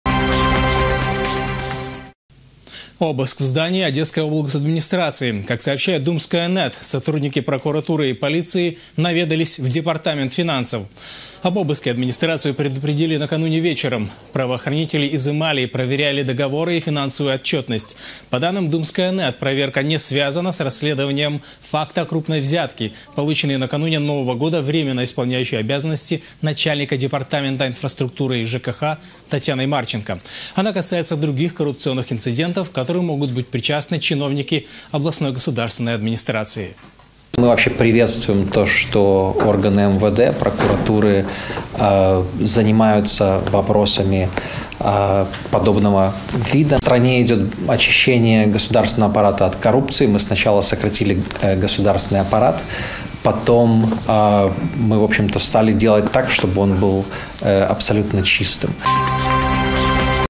Обыски в ОГА
Обыск в здании Одесской облгосадминистрации. Как сообщает dumskaya.net, сотрудники прокуратуры и полиции наведались в департамент финансов.